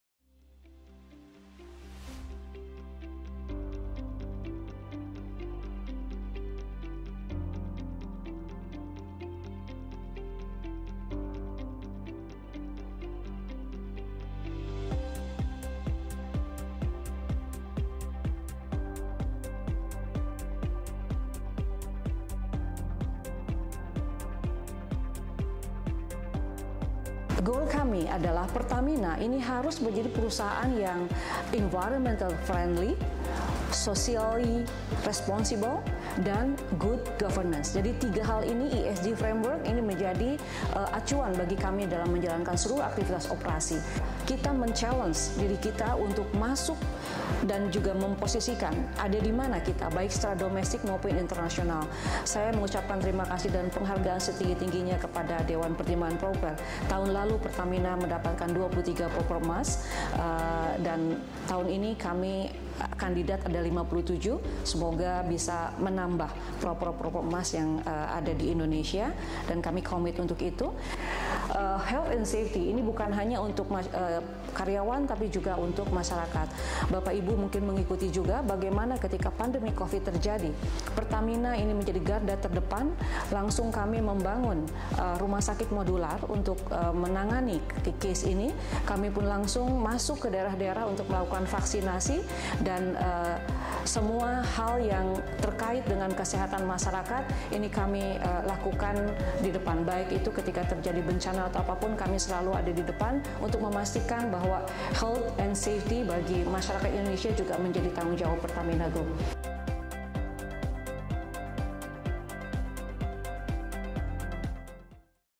Cuplikan Presentasi Direktur PT Pertamina (Persero)
[PRESENTASI CEO PT PERTAMINA (PERSERO)]

Halo SobatHijau🍃

Yuk, kita dengarkan cuplikan singkat Presentasi Ibu Nicke Widyawati, CEO PT Pertamina (Persero).

SobatHijau, yuk simak video berikut ini ya.